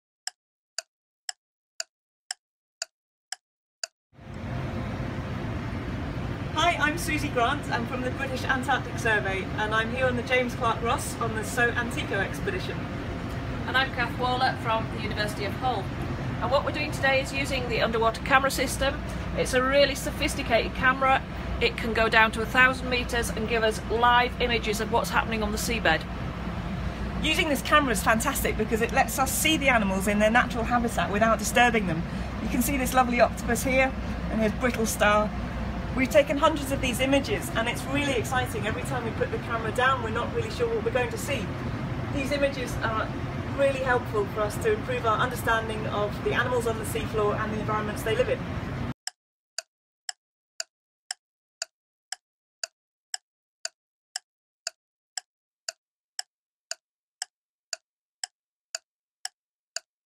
Polar Science in 60 Seconds | Sea Bed Camera System
We’ve challenged our science, engineering and support staff and their research collaborators to explain in 60 seconds what they do. Susie Grant and Cath Waller talk us through the underwater camera systems British Antarctic Survey uses to monitor live activity on the sea bed.